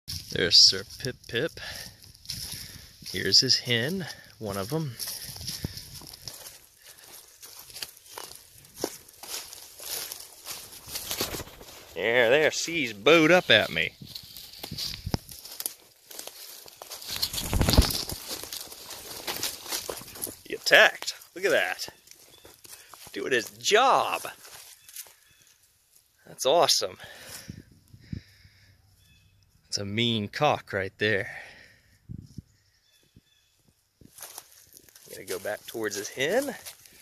SirPipPip Attacks
Thoroughbred Welsummer cock unleashes fury.